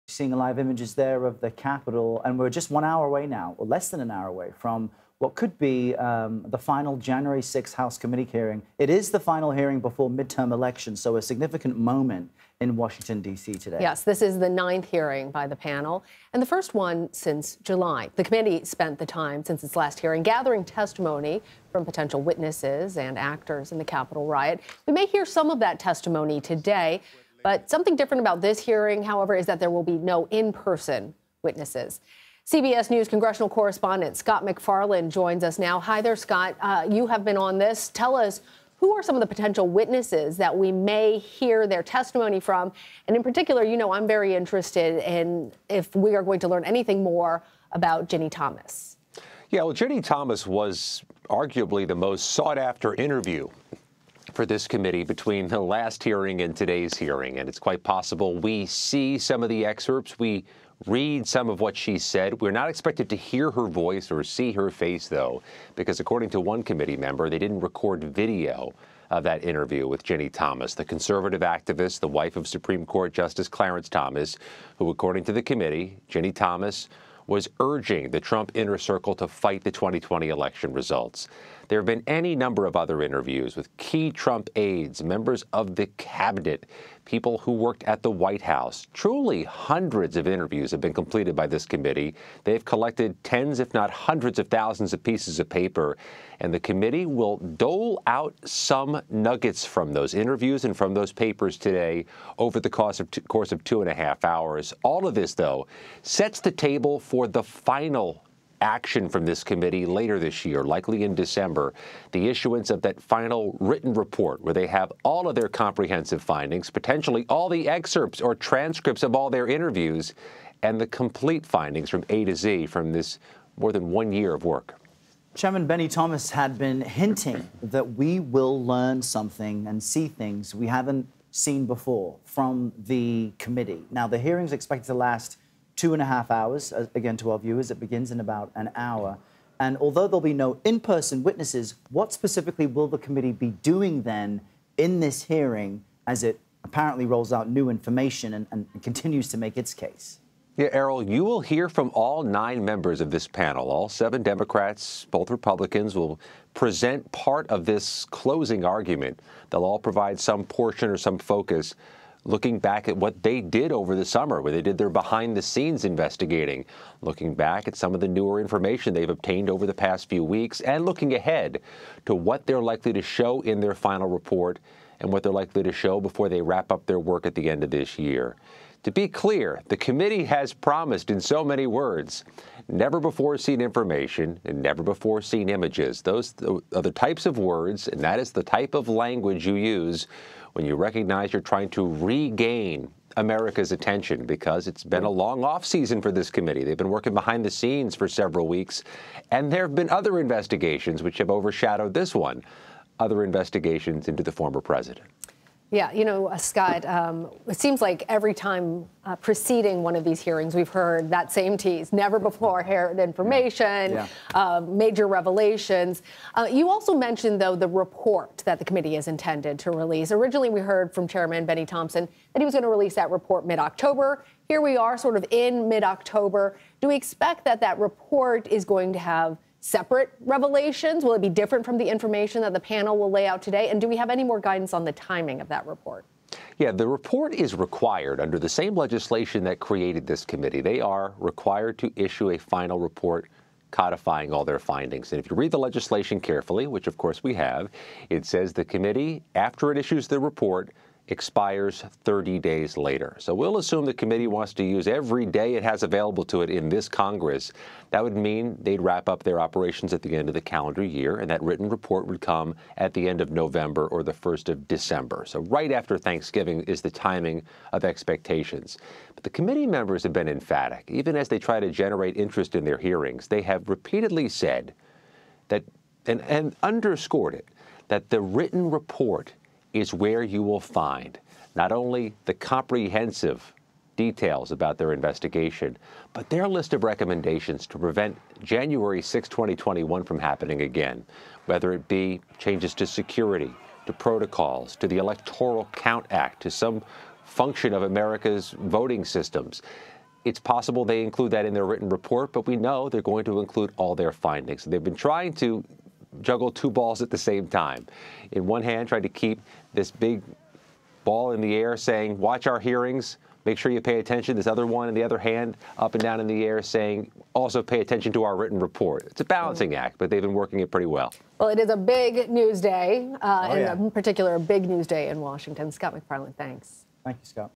January 6 committee holding hearing after interviewing conservative activist Ginni Thomas
The January 6 House select committee is holding its ninth public hearing Thursday. CBS News congressional correspondent Scott MacFarlane discussed what the panel is expected to share, including potential testimony from Ginni Thomas.